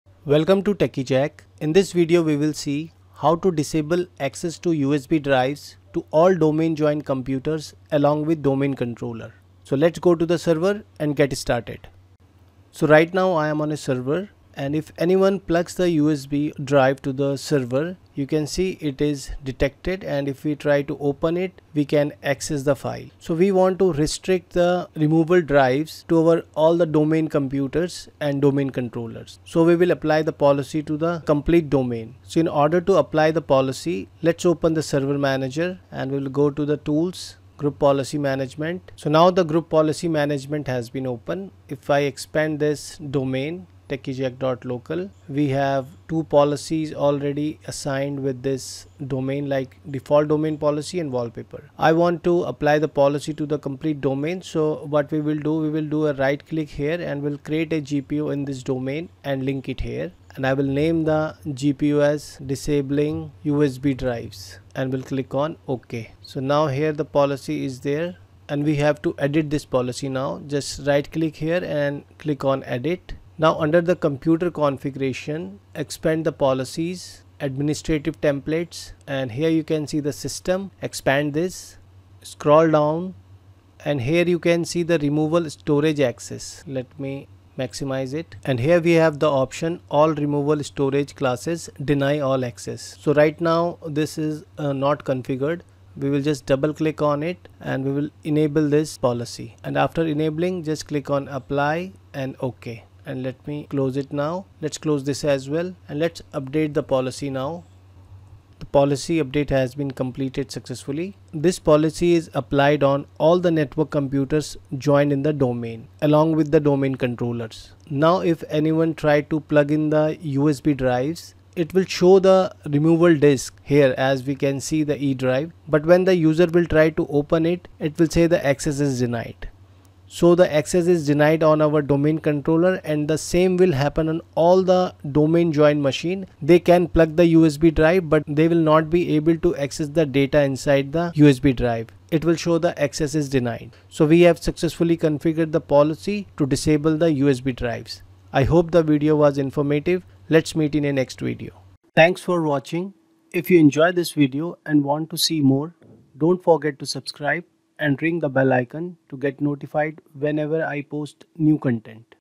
How to Block USB Drives with Group Policy | Server 2025
Learn how to block USB drives using Group Policy in Windows. Protect your data and improve system security with this step-by-step guide for IT admins and users. Works on Windows 10 & 11.

In this video of how to block USB drives with group policy in Windows Server 2025.

You will learn to block the USB devices for all users and computers via group policy.

This video helps you configure the group policy to block USB mass storage on all domain-joined machines.

I assure you that, after watching this video completely, you will be able to block the USB drives using group policy for your Active Directory environment.